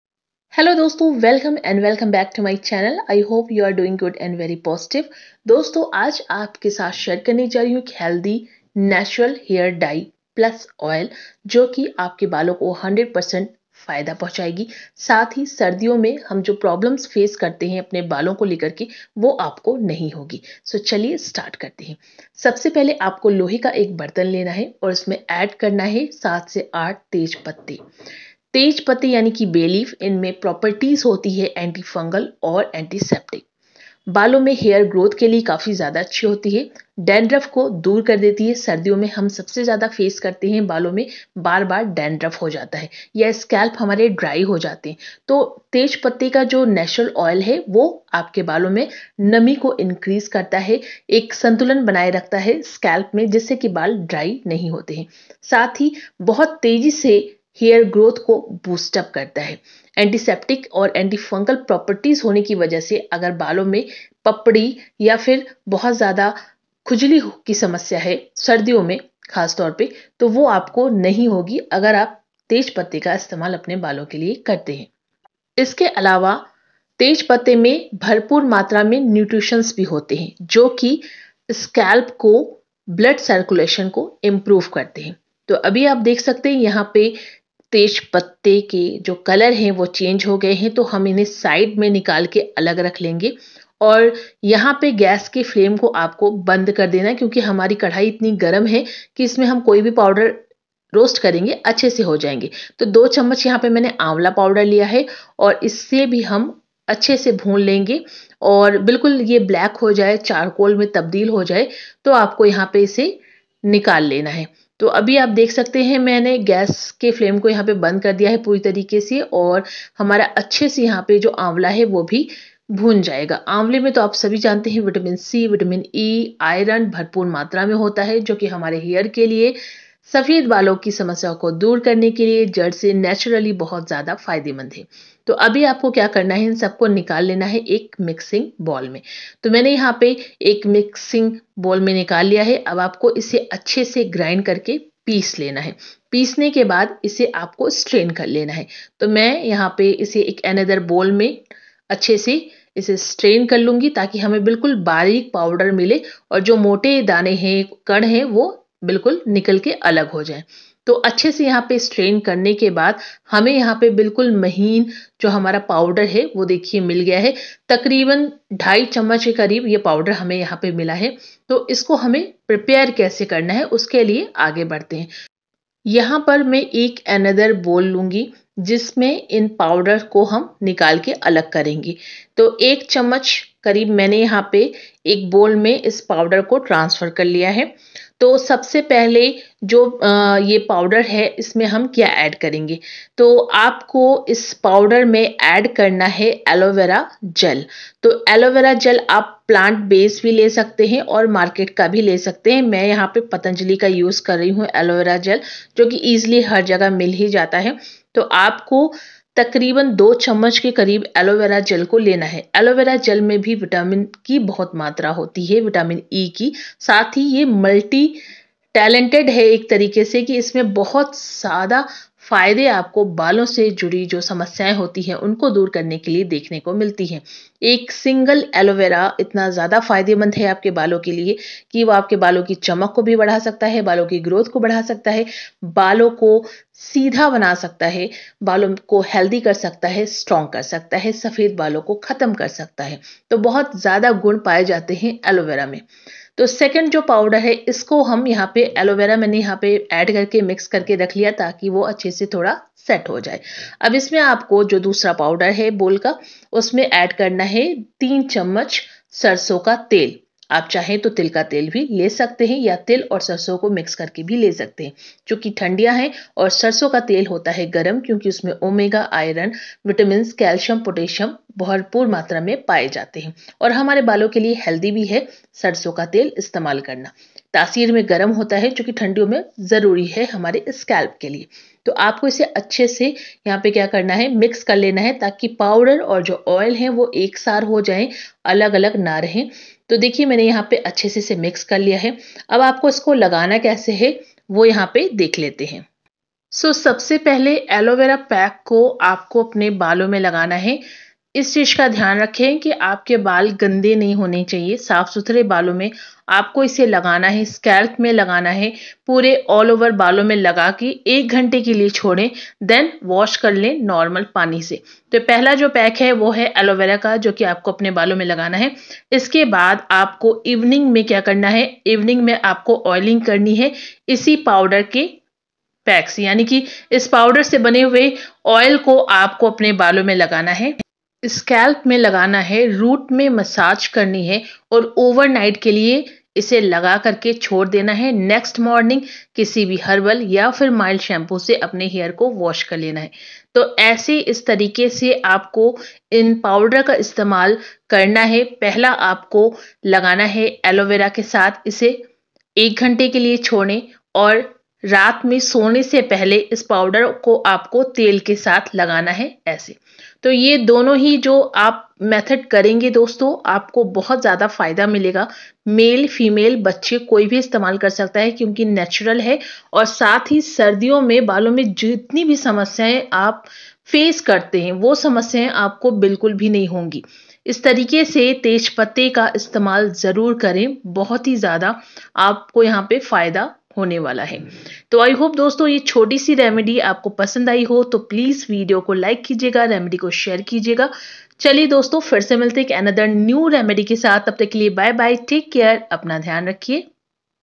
White Hair Turn Instant Black/Without Heena Dye/ Safed Balon Ka Gharelu upay
सफ़ेद बाल तुरंत काले करें/हिना डाई के बिना/Safed Balon Ka gharelu upay

Coffee में 2 पाउडर मिलाकर लगालें सफेद बाल होंगे ही नहीं। Without Heena Dye| Safed Balo ka pakka ilaj

royal ruchi hair dye
royal ruchi black hair